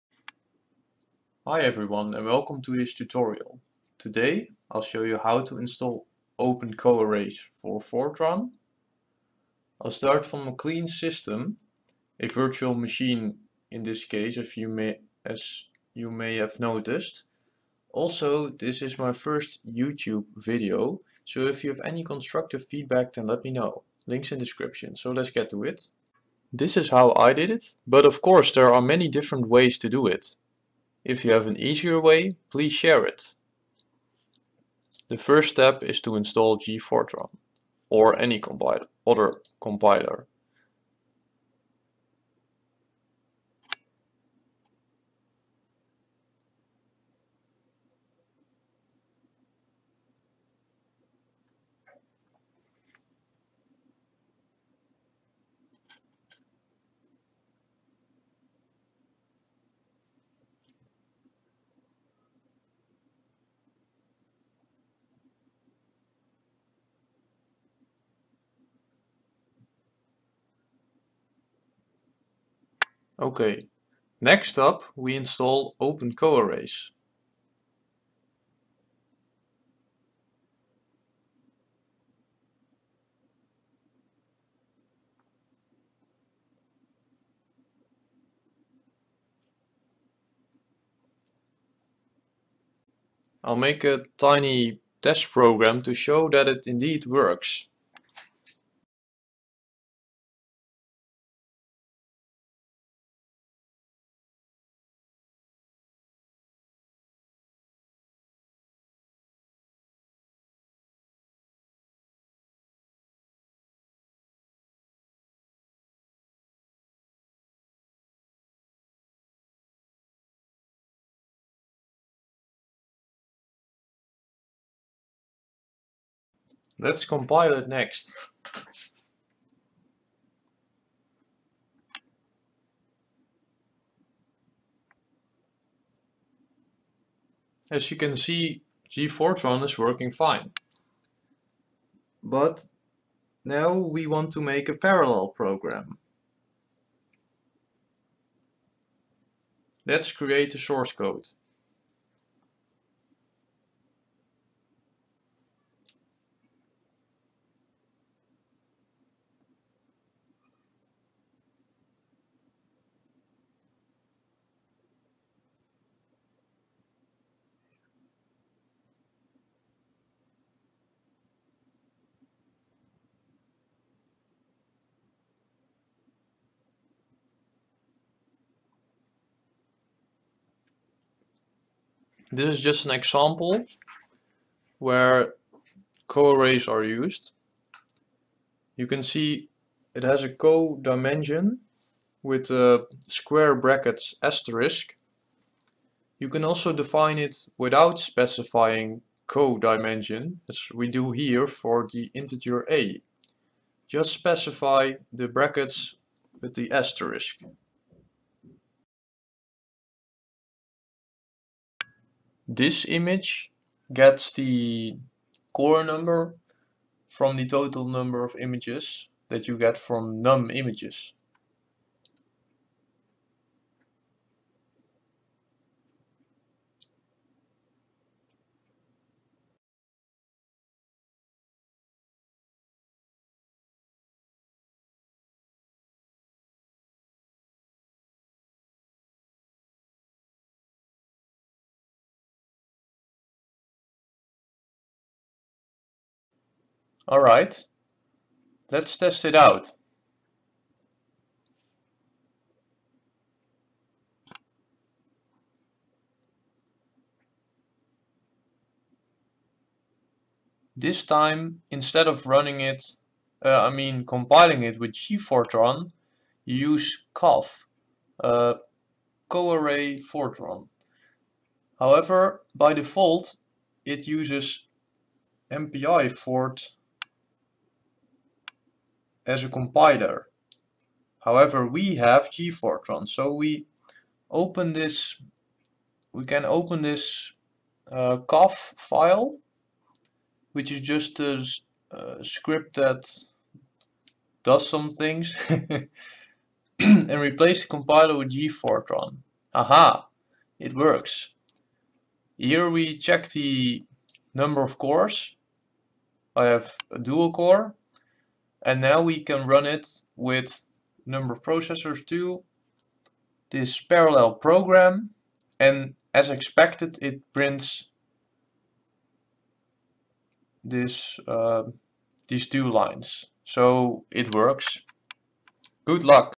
Coarrays in Fortran (CAF) tutorial, part 1: Setup
I will guide you through the setup of opencoarrays on a clean system.
Constructive feedback & questions are welcome.

Note: last command I used was 'cafrun -np 2 ParallelProgram.exe', but 'mpiexec -n 2 ParallelProgram.exe' also works fine.